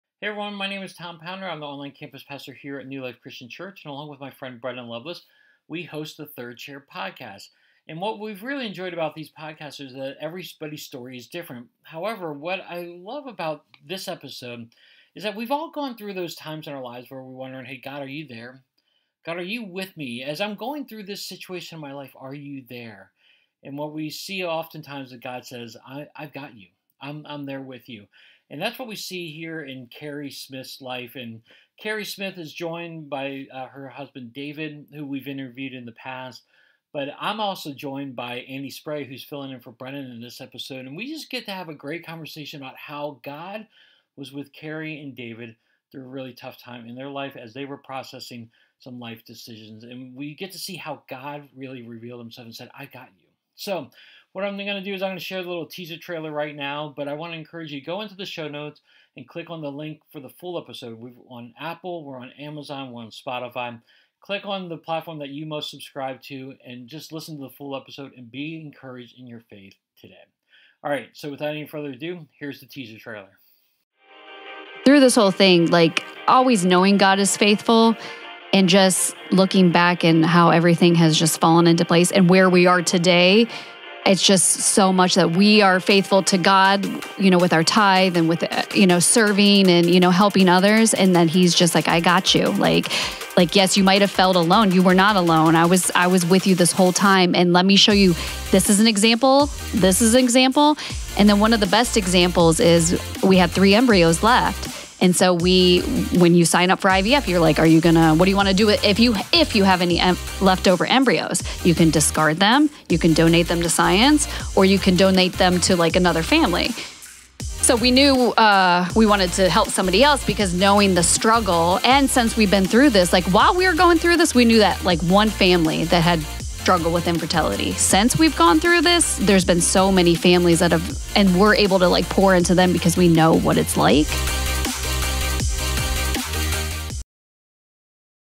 I've Got You (Third Chair Podcast Teaser)
This week, Tom is joined by Andy Spray and they talk to Keri and David Smith about how God was with them during a very challenging time in their life. Hear Keri's story and what God is doing in their life today.